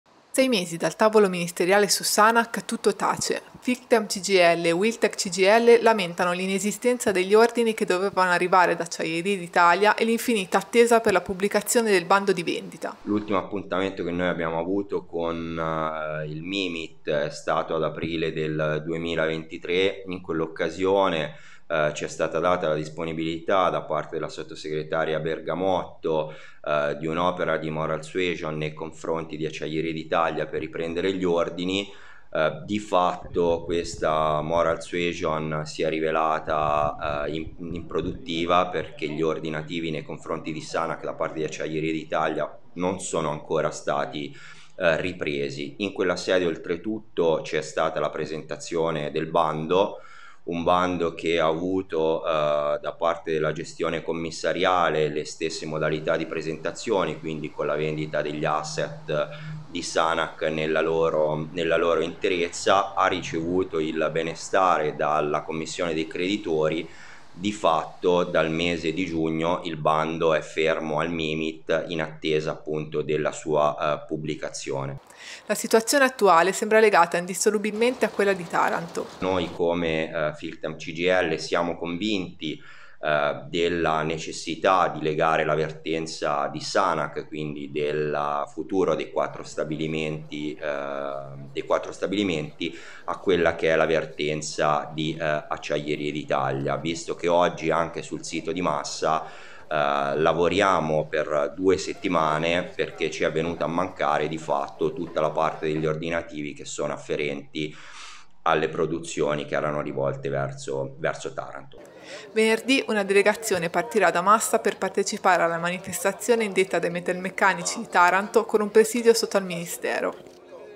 Sanac, ancora tutto fermo: presidio venerdì al Ministero
Rabbia e delusione tra gli operai della Sanac di Massa: l'azienda siderurgica specializzata nella produzione di refrattari da anni in amministrazione straordinaria e in attesa di una mossa da parte del Governo. Pronta una nuova mobilitazione.